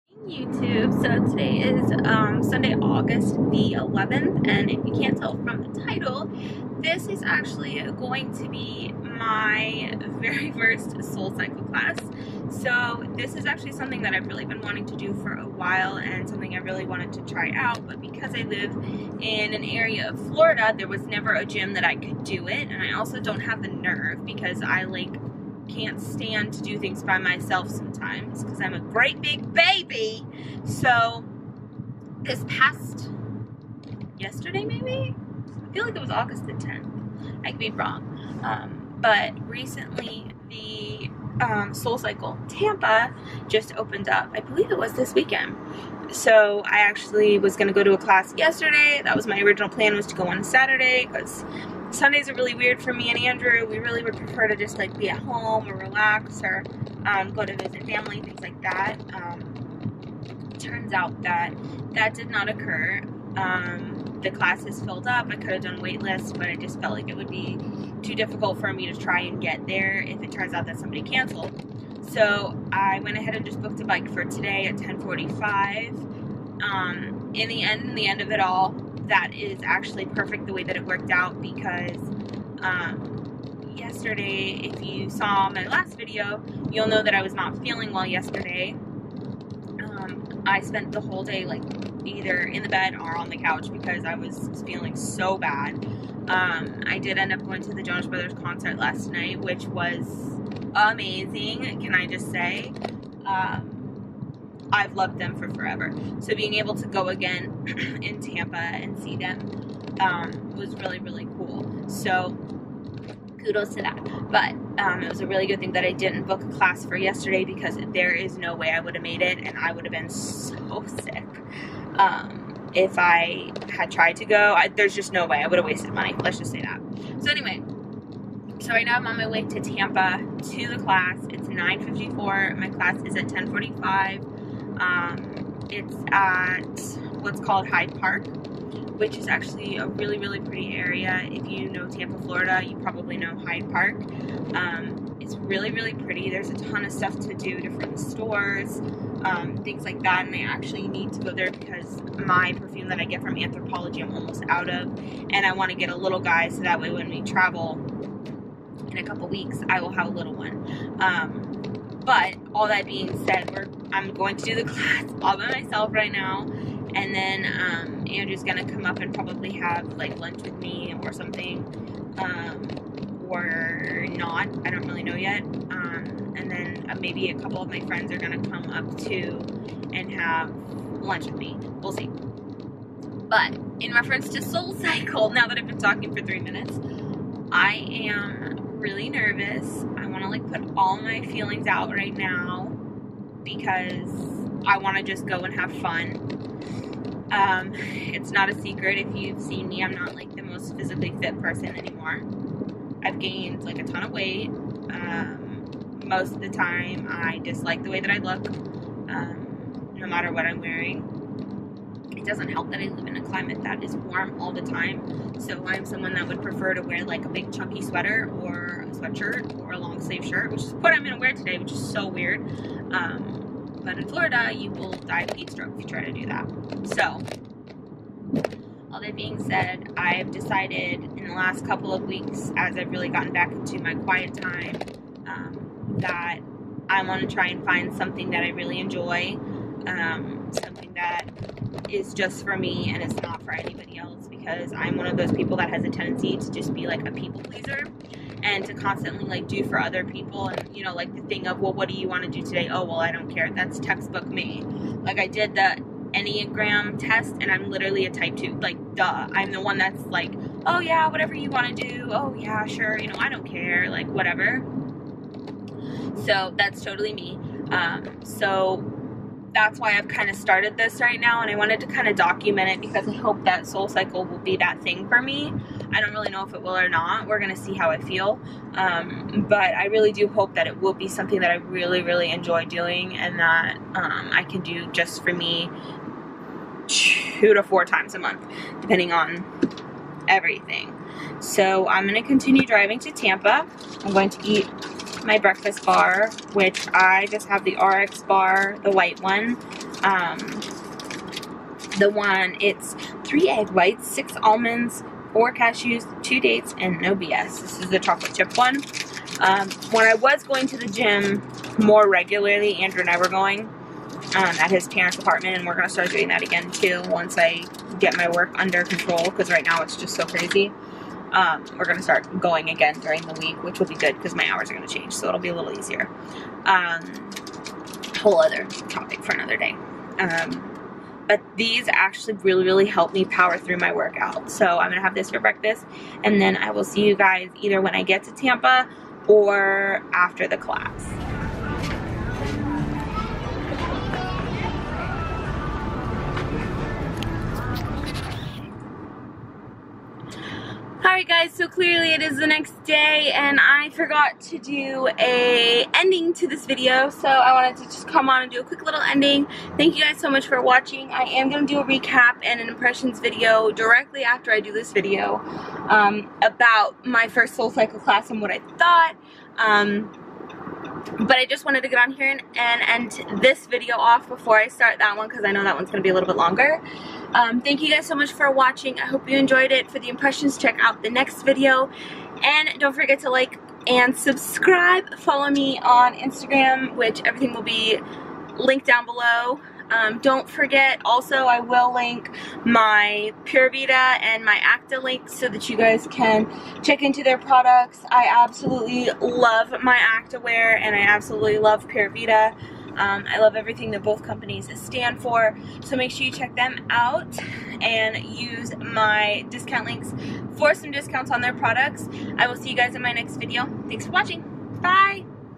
Why I Started SoulCycle
Hello Youtube!
Today's video is a bit about why I decided to start doing Soul Cycle.
My next video will be about my impressions following my first class.

I'm so excited to be a Brand Ambassador for two AMAZING companies that I love and am so proud to support.

HILARYHARTOG20

hilgrace15

Equipment Used:
Canon G7X